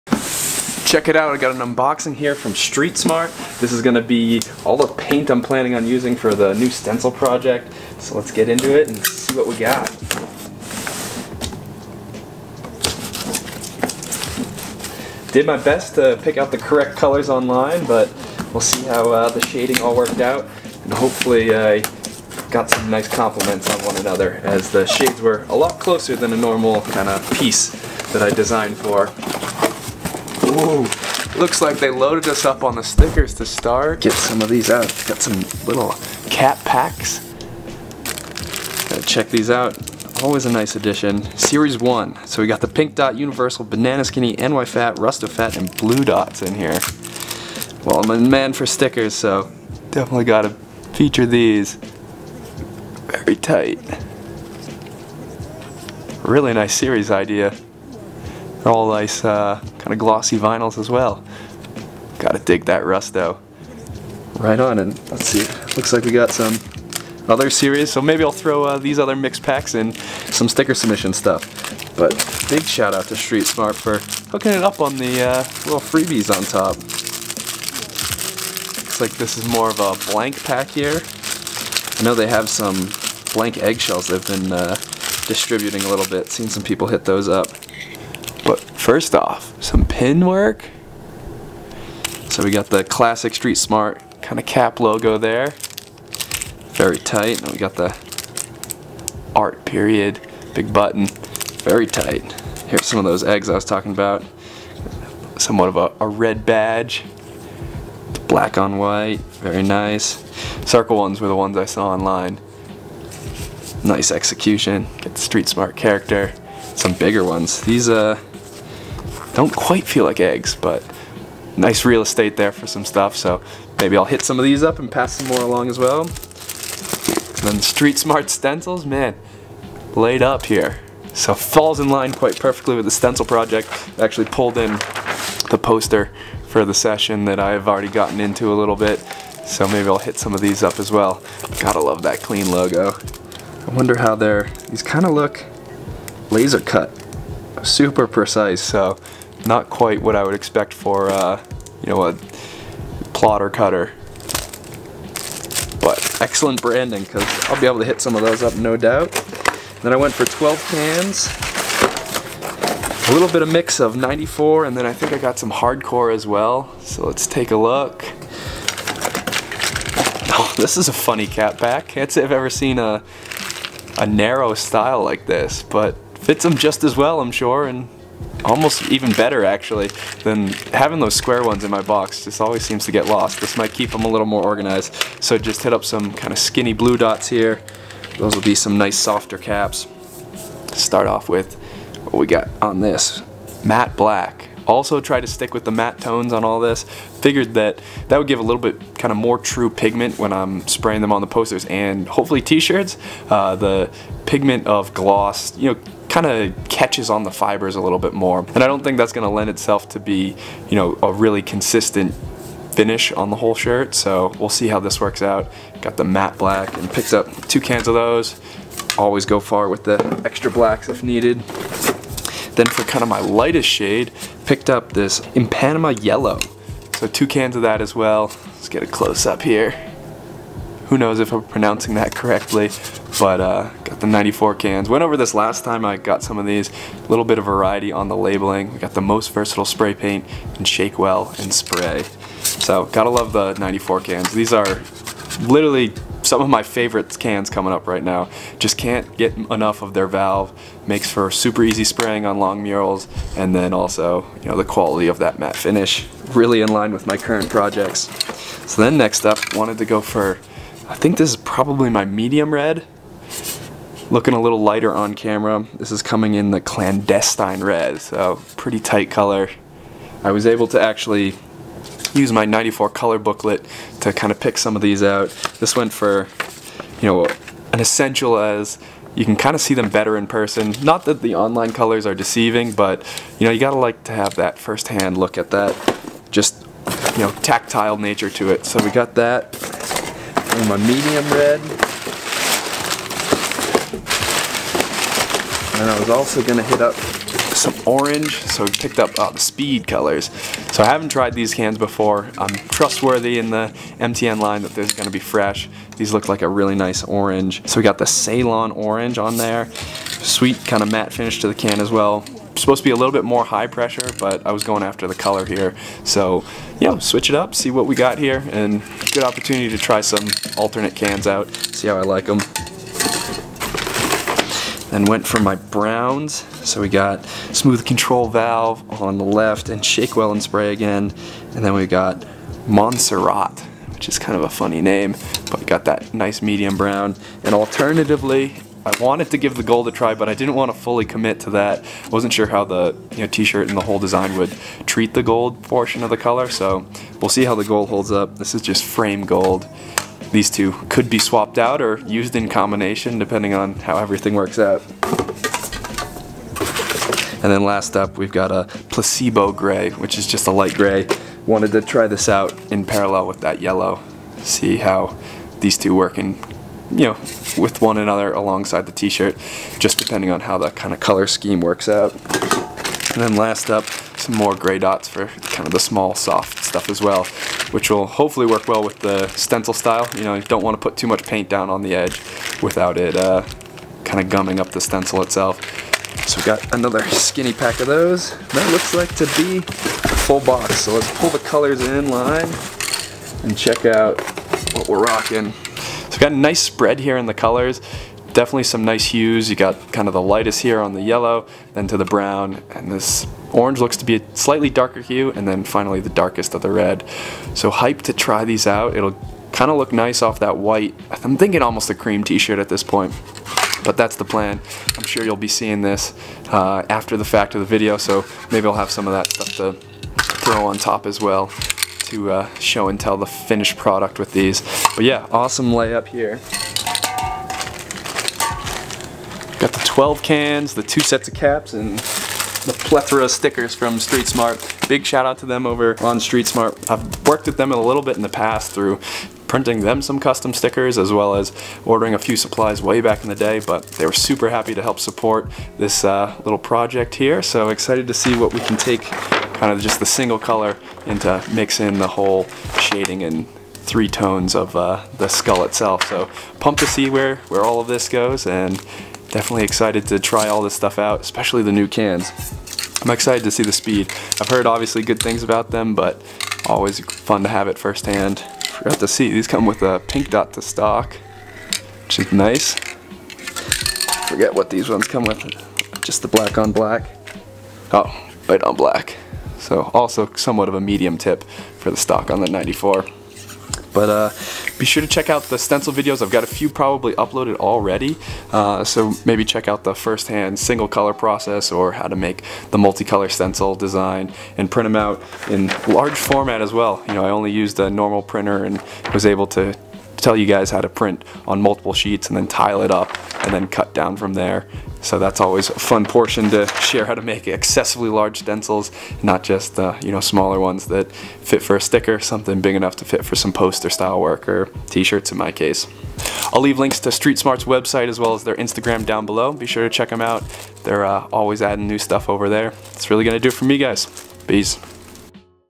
MTN SPRAY PAINT Unboxing From Street Smart Net
Eager to take these cans and use them for my skull stencil project. Be on the look out for the mutli color session next week. Check out the supplies from Street Smart

KL / Plndr Rep Code SIVE